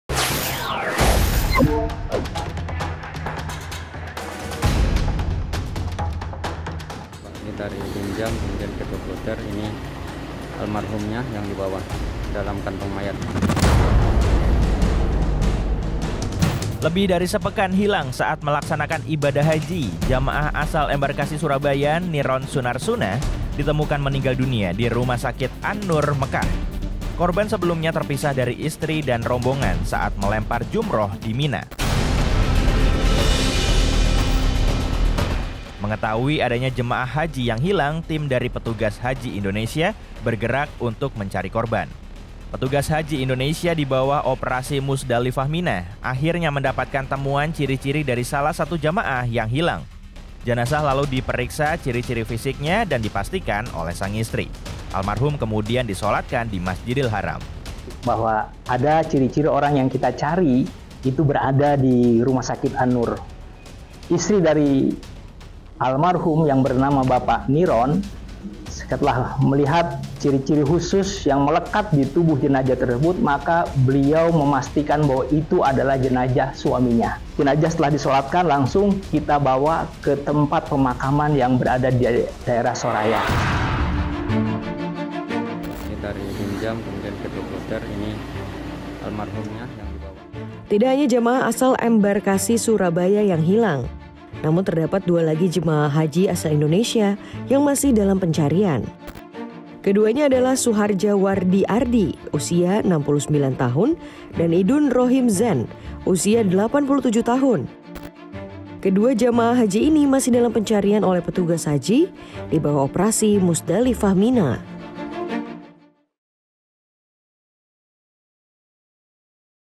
Hilang saat Ibadah Haji, Jemaah ini Ditemukan Meninggal Dunia | POP NEWS
Setelah hilang lebih dari sepekan, jemaah haji Niron Sunar Sunah ditemukan meninggal dunia di Rumah Sakit An Noor Mekkah.

Sebelumnya, korban terpisah dari istri dan rombongan saat melempar Jumrah di Mina.

Tim petugas haji dari Indonesia bergerak mencari korban saat dikabarkan adanya rombongan haji yang hilang.

Di bawah operasi Muzdalifah di Mina, tim petugas haji Indonesia akhirnya mendapatkan temuan ciri-ciri salah satu jemaah yang hilang.

Jenazah lalu diperiksa ciri-ciri fisiknya dan dipastikan oleh istri. Almarhum kemudian disholatkan di Masjid al-Haram dan dibawa ke tempat Pemakaman Soraya.